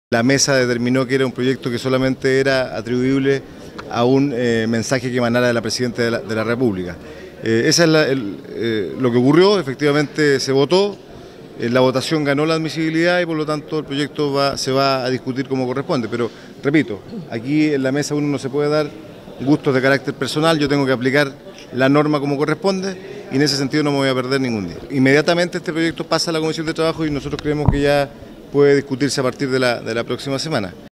Presidente de la Cámara explicó negativa de la Mesa para legislar sobre rebaja de jornada laboral
El presidente de la Cámara de Diputados, Fidel Espinoza señaló que la Mesa consideró inadmisible el proyecto presentado por la diputada Camila Vallejo para rebajar a 40 horas semanales la jornada laboral,  por tratarse de una materia propia del Ejecutivo, por lo cual el proyecto debió ser sometido a votación de Sala, la cual terminó por respaldar la admisibilidad y enviar el proyecto a la comisión de Trabajo, para iniciar el trámite legislativo.